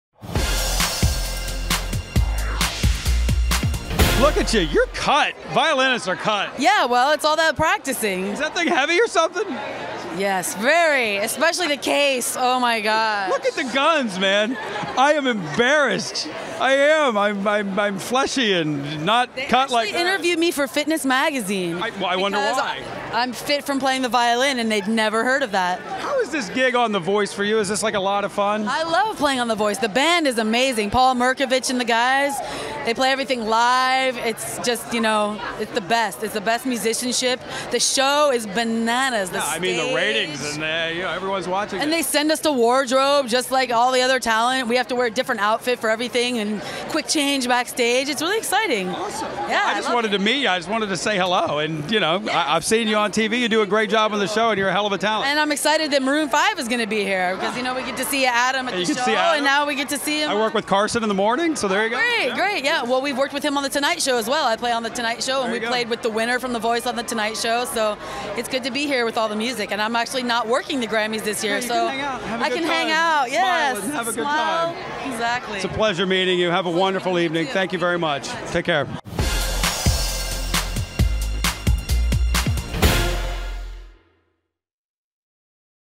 55th Grammy Awards - Christine Wu Interview
Christine Wu red carpet interview with Chris Booker at the 55th Annual GRAMMY Awards.